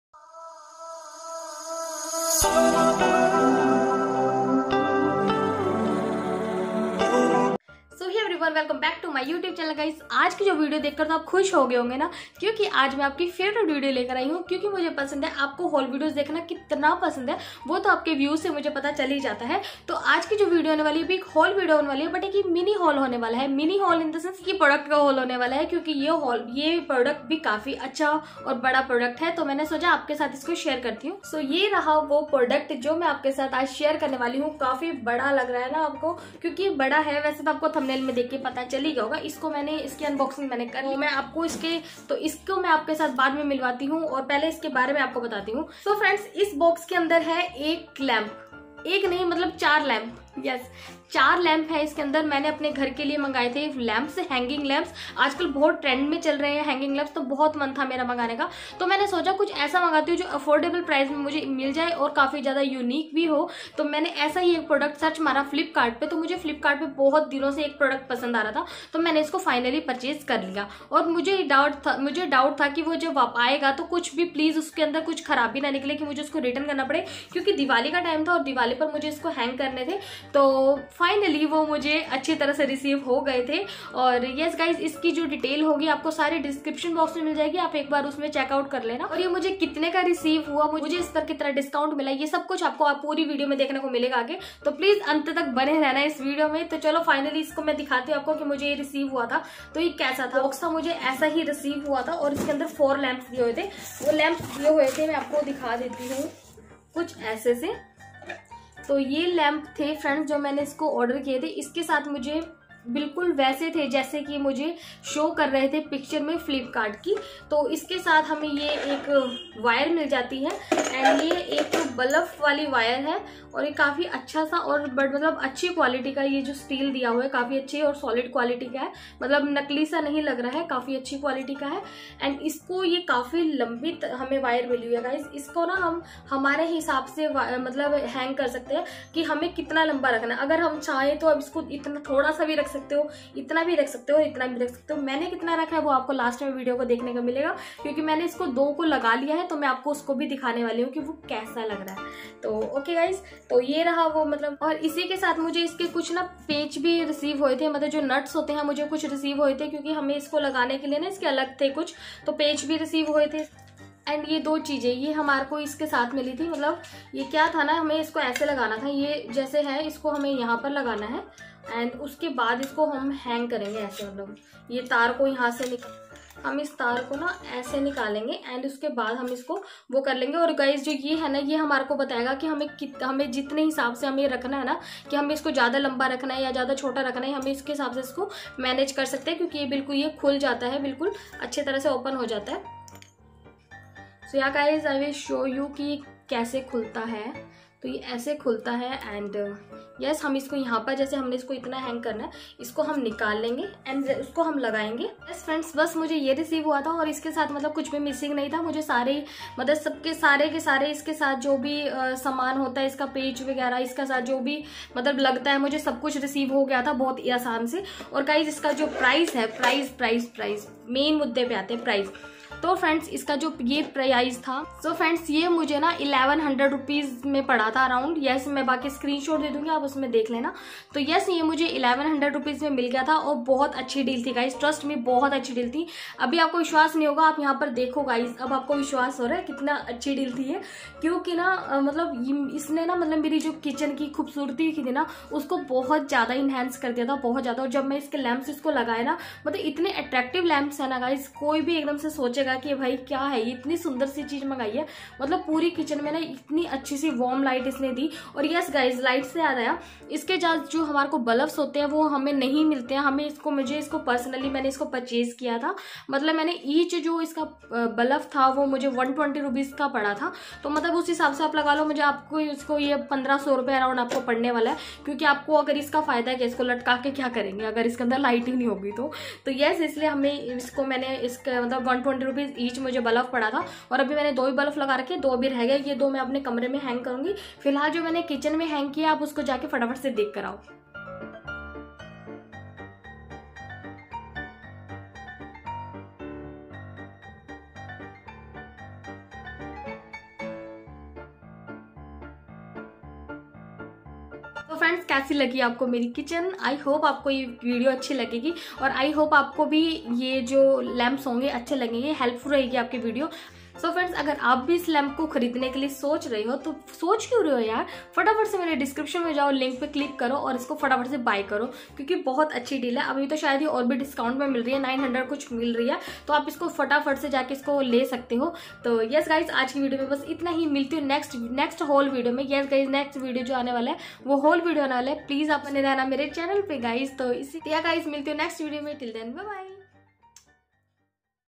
Cheapest Flipkart Mini Haul Hangling Lamps🏮
Cheapest Flipkart Mini Haul Under Rs. 990/-Hangling Lamps🏮

Product Link:- Take a look at this Areezo Wooden Square Flower Design hanging lamp (Set of 4),bulb not included Pendants Ceiling Lamp on Flipkart

if u like this video so pls

bieee❤️